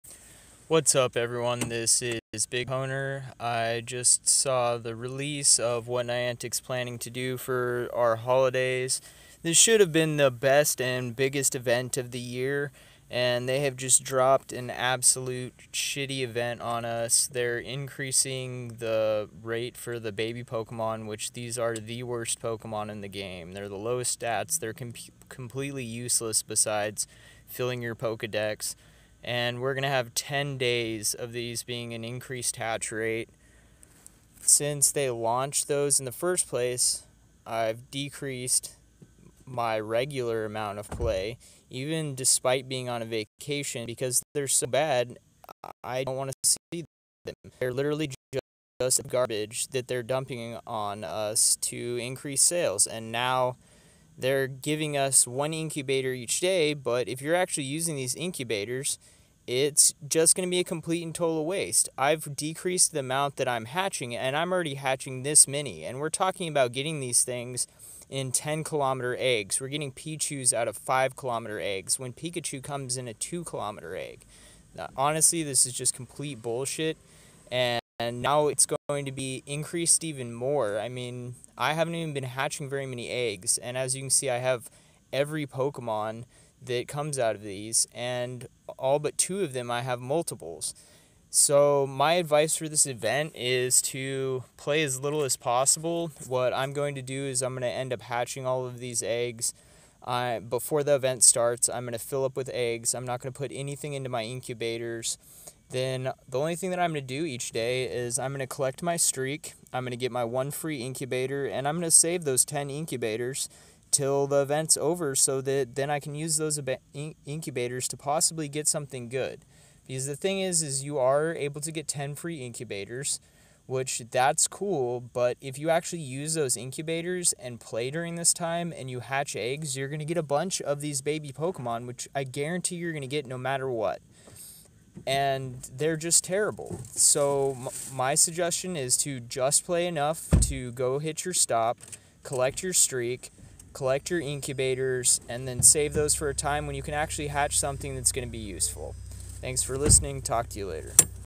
Christmas event.... Niantic gives us coal and expects money in return
Horrible event, they dump even more garbage on us, here's a way to actually get something out of it.